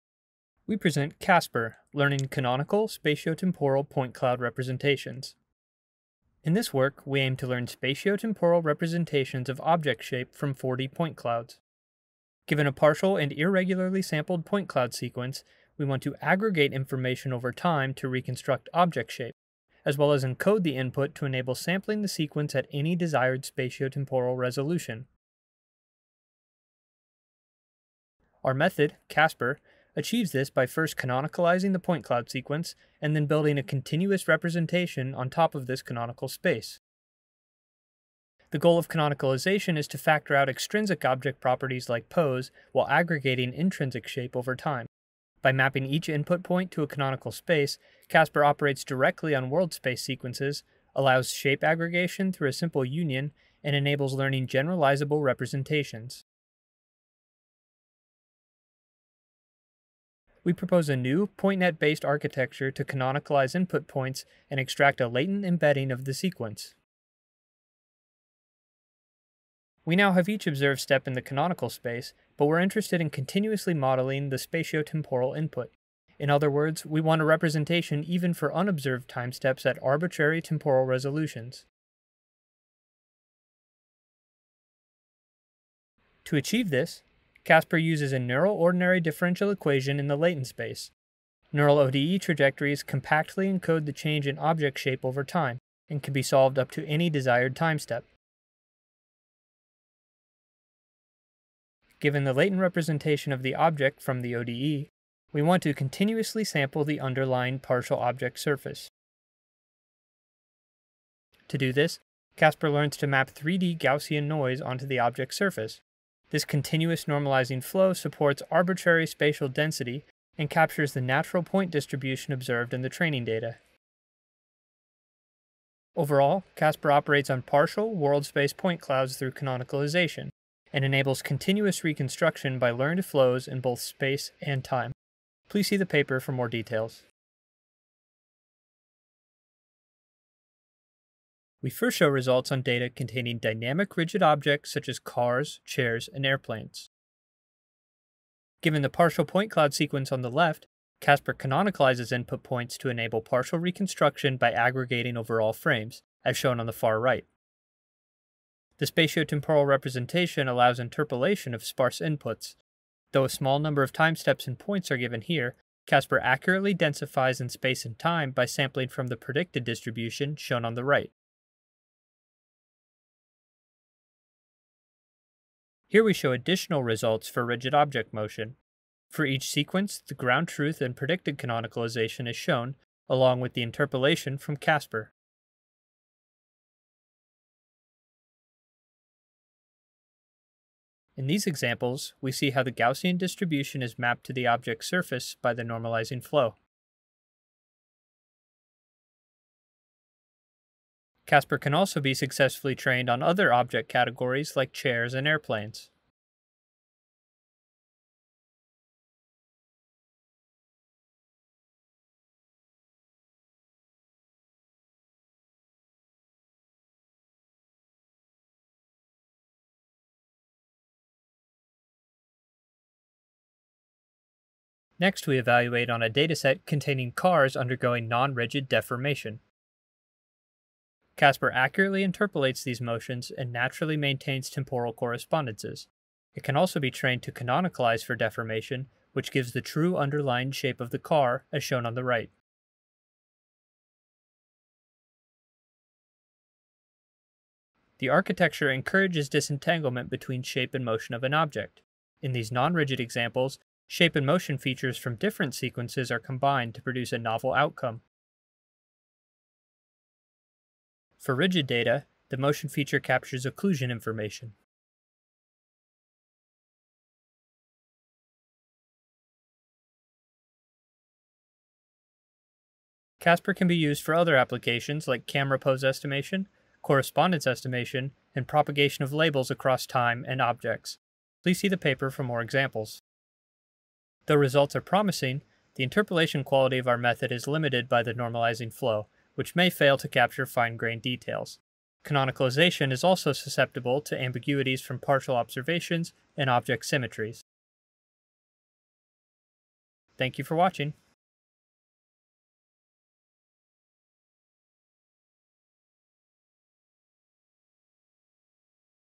CaSPR: Learning Canonical Spatiotemporal Point Cloud Representations (NeurIPS 2020)
Presented at Advances in Neural Information Processing Systems (NeurIPS) 2020 [Spotlight].

Abstract: We propose CaSPR, a method to learn object-centric Canonical Spatiotemporal Point Cloud Representations of dynamically moving or evolving objects. Our goal is to enable information aggregation over time and the interrogation of object state at any spatiotemporal neighborhood in the past, observed or not. Different from previous work, CaSPR learns representations that support spacetime continuity, are robust to variable and irregularly spacetime-sampled point clouds, and generalize to unseen object instances. Our approach divides the problem into two subtasks. First, we explicitly encode time by mapping an input point cloud sequence to a spatiotemporally-canonicalized object space. We then leverage this canonicalization to learn a spatiotemporal latent representation using neural ordinary differential equations and a generative model of dynamically evolving shapes using continuous normalizing flows. We demonstrate the effectiveness of our method on several applications including shape reconstruction, camera pose estimation, continuous spatiotemporal sequence reconstruction, and correspondence estimation from irregularly or intermittently sampled observations.